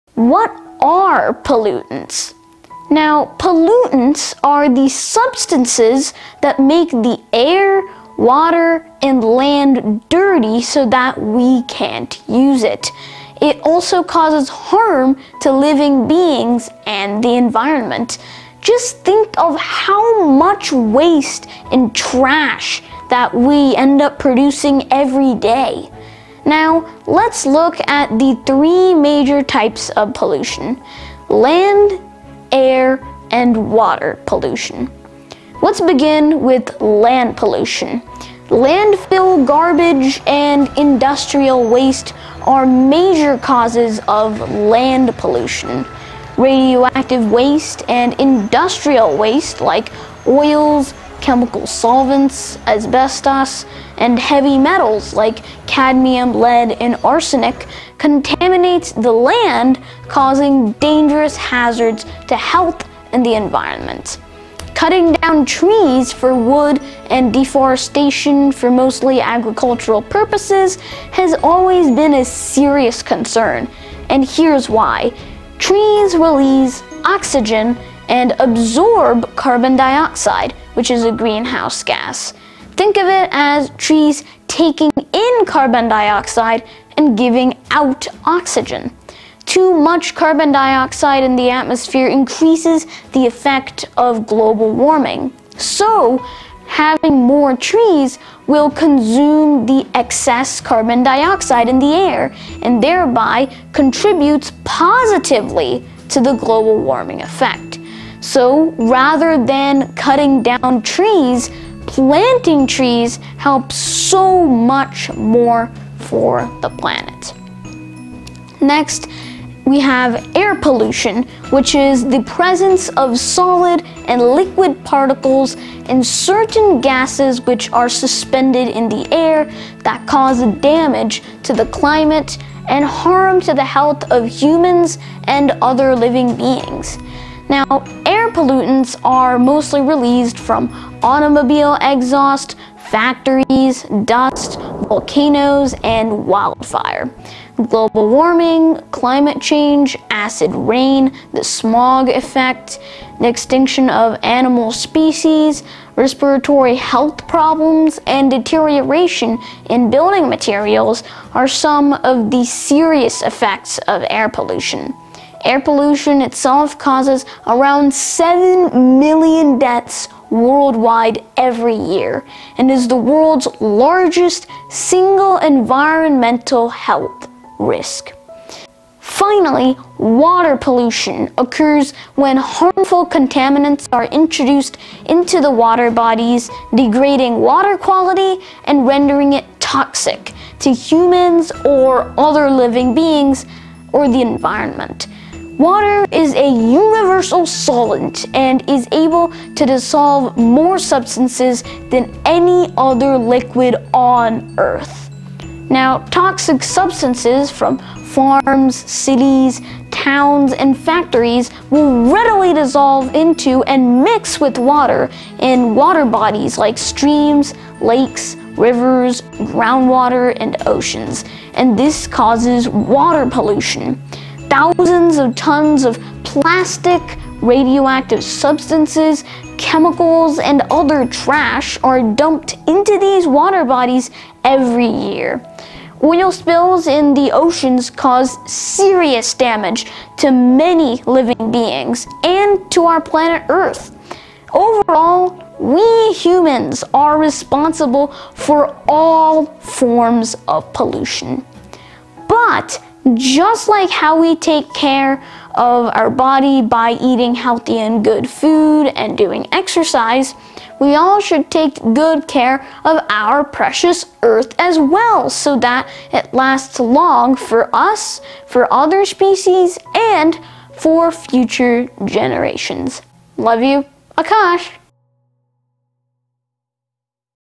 What is Pollution? |Types of Pollution | Land | Air | Water
This video explains what pollution is and the three most common types of pollution. Minimizing pollution will help to save our planet!